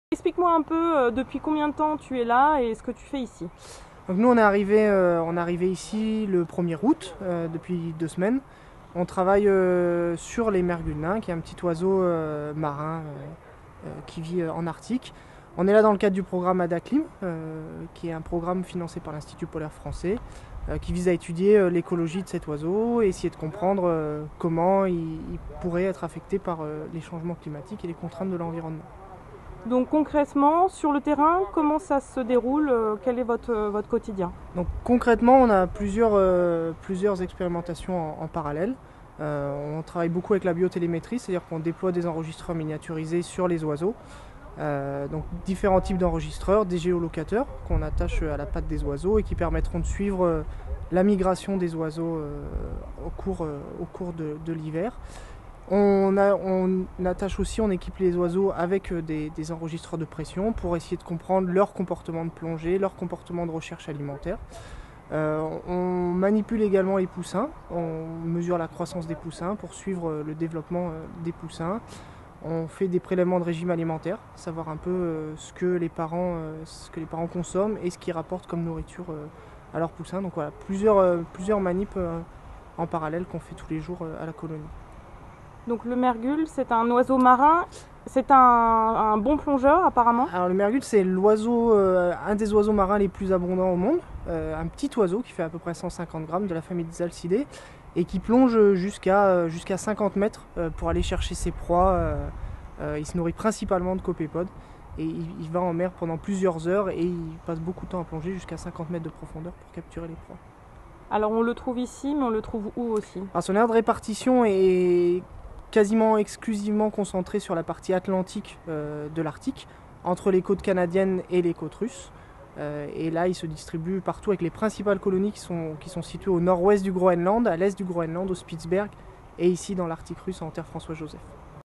Interview de Jérôme Fort, écologue marin - Expédition Tara Oceans Polar Circle - 11 août 2013
Jérôme Fort, écologue marin, nous explique l'étude menée sur les mergules nains.